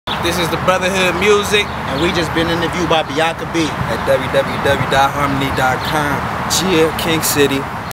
The Brother-Hood Music Interviews with BiancaBee.com
The Brother-Hood Music Interviews with Bianca Bee to talk about the future of their group & how they started.